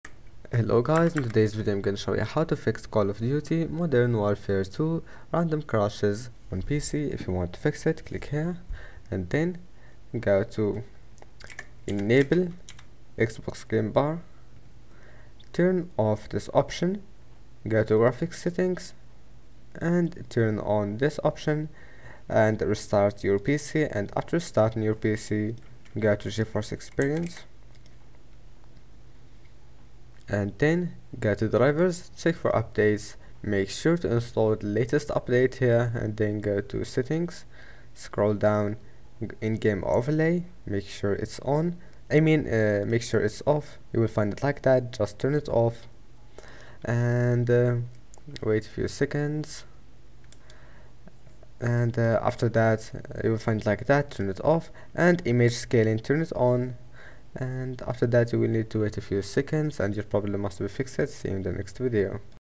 How to Fix COD MW2 All Crashes | Call of Duty Modern warfare 2 Crashing | Easy Ways to Solve
in this video we discuss :